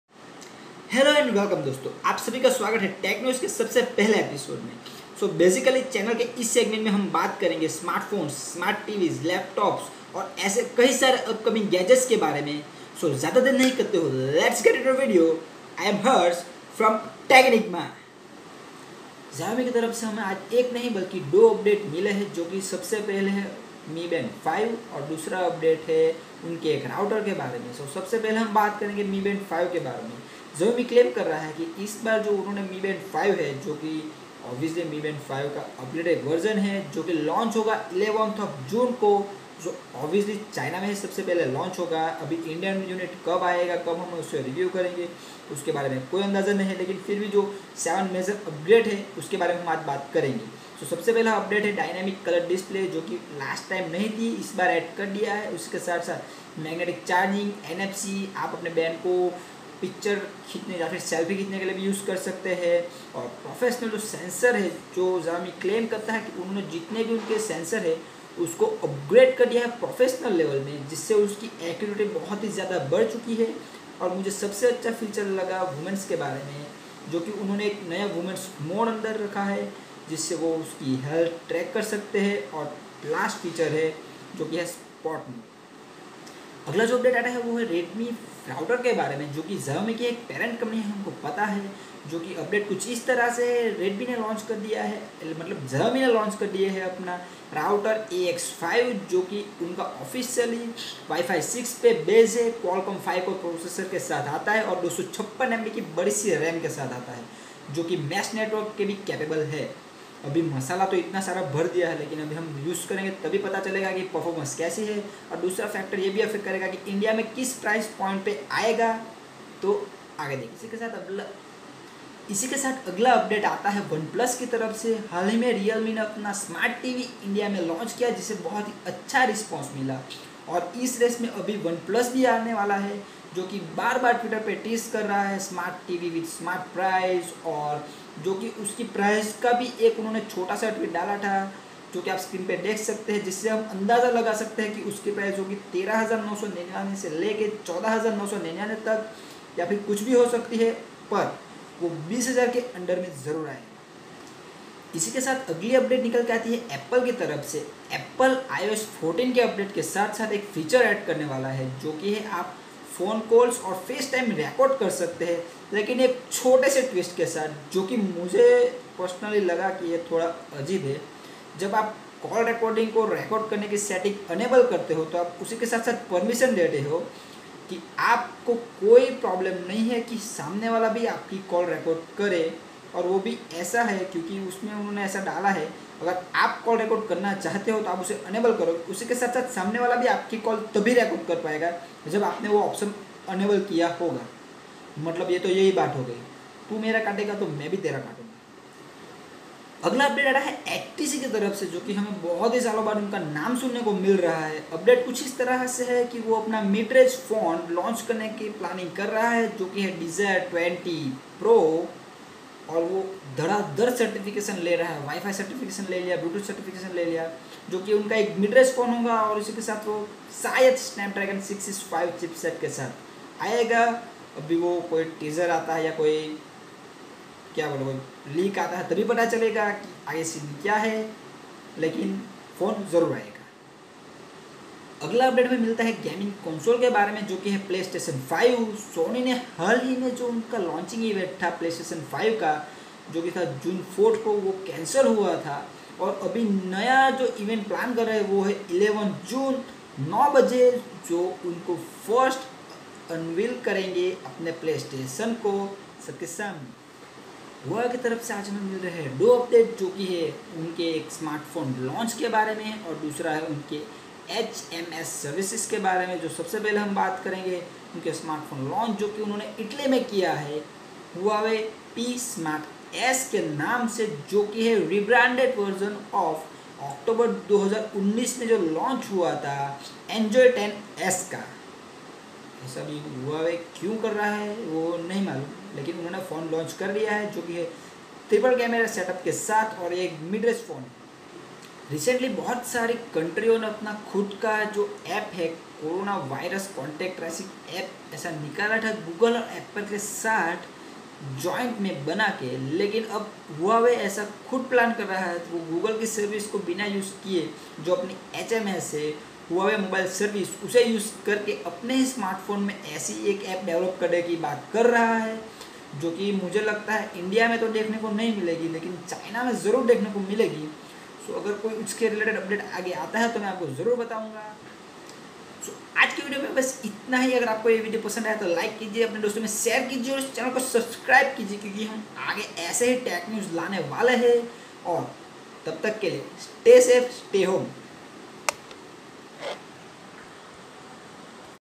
#1 Tech News | Mi band 5,Oneplus smart tv,Huawei HMS ,P Smart S,IOS 14 etc..
sabhi ka swagat he tech news ke sabse pehle episode me
..thod to nai lekin aj video upload karne me ku6 juada der ho gai he ..fir bhi mehnat ki he to uplod to jarur karenge ..aj se humare chanel pe tech news ka segment start ho chuka he so jaldi se subscribe kar lo ...
video me bat hu a aj ke updates
jo je
mi band 5
oneplus smart tv in india
apple Ios 14
sony playstation 5 update
Huawei p smart s aur hms
aur bhi bahut sari garma garam tech news ...

peace ✌